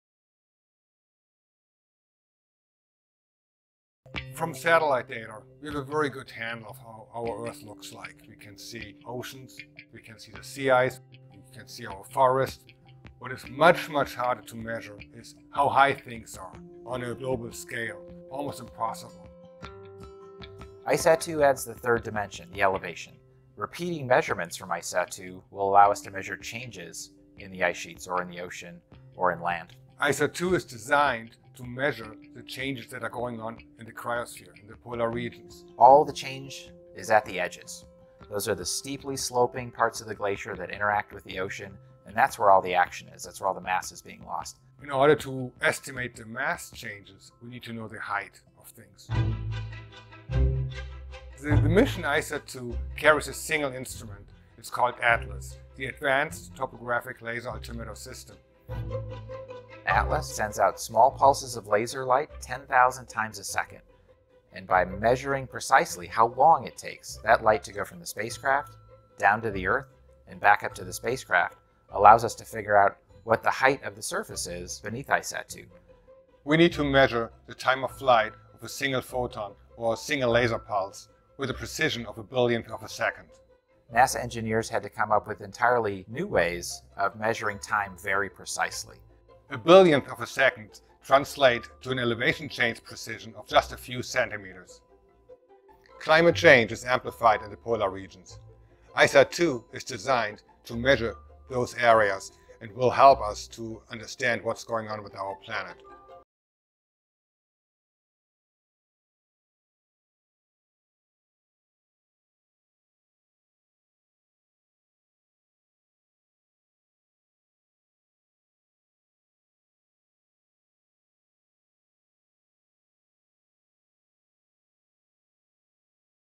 ICESat-2 explained
NASA’s ICESat-2, the Ice, Cloud and land Elevation Satellite-2, will provide critical observations of how ice sheets, glaciers and sea ice are changing. ATLAS (Advanced Topographic Laser Altimeter System), the single instrument on ICESat-2, will measure melting ice sheets and investigate how this effects sea level rise, investigate changes in the mass of ice sheets and glaciers, estimate and study sea ice thickness, and measure the height of vegetation in forests and other ecosystems worldwide.